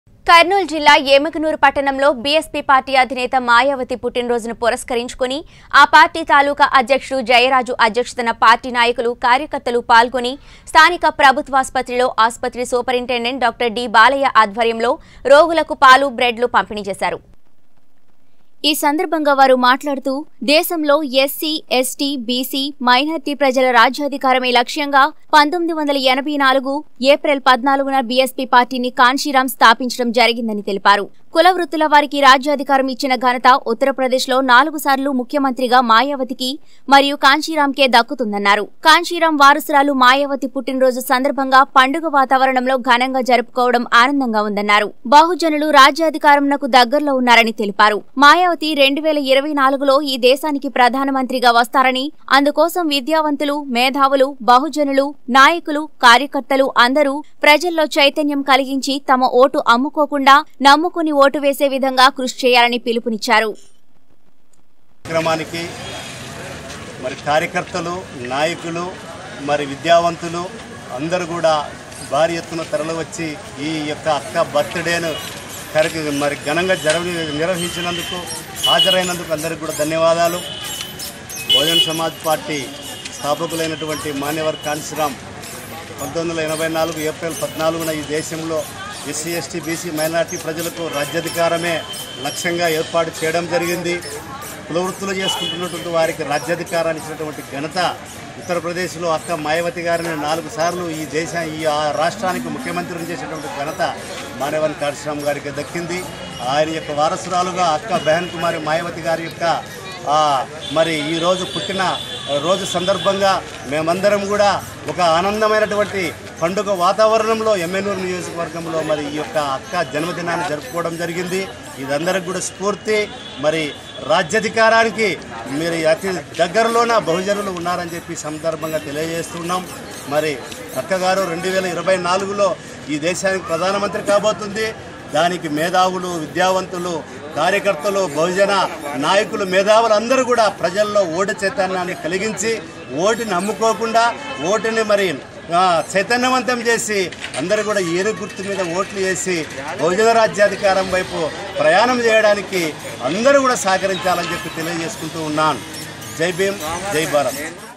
బీఎస్పీ పార్టీ అధినేత మాయావతి పుట్టినరోజు పురస్కరించుకొని రోగులకు పాలు బ్రెడ్ పంపిణీ
బీఎస్పీ పార్టీ అధినేత మాయావతి పుట్టినరోజు పురస్కరించుకొని డాక్టర్ డి బాలయ్య ఆధ్వర్యంలో రోగులకు పాలు బ్రెడ్ పంపిణీ చేసిన నాయకులు కార్యకర్తలు కర్నూలు జిల్లా ఎమ్మిగనూరు | 19 Newstelugu